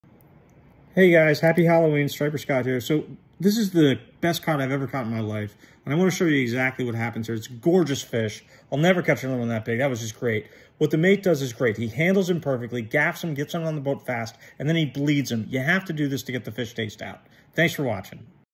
How to bleed a cod
Happy Halloween!  I'm sorry if this is gruesome, but, to properly express a fish after catch is essential, it's less painful for the fish and it gets the blood out of the meat.  Great day on the water!